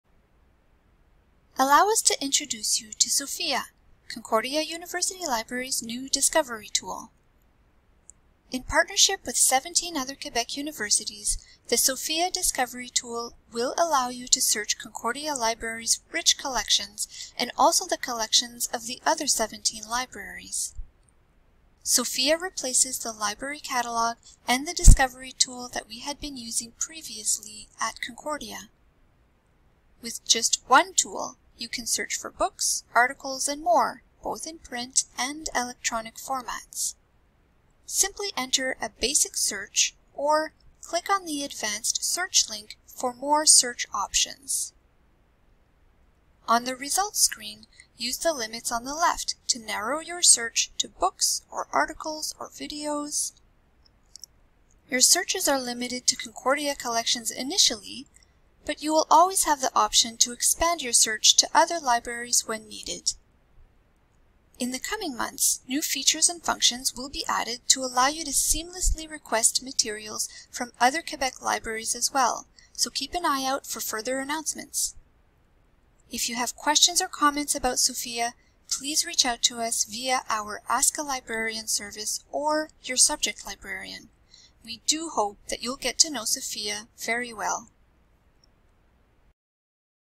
Introduction to the Sofia Discovery tool
In partnership with 17 other Quebec universities, the Sofia Discovery tool will allow you to search Concordia University Library’s rich print and electronic collections, and also the collections of the other 17 libraries.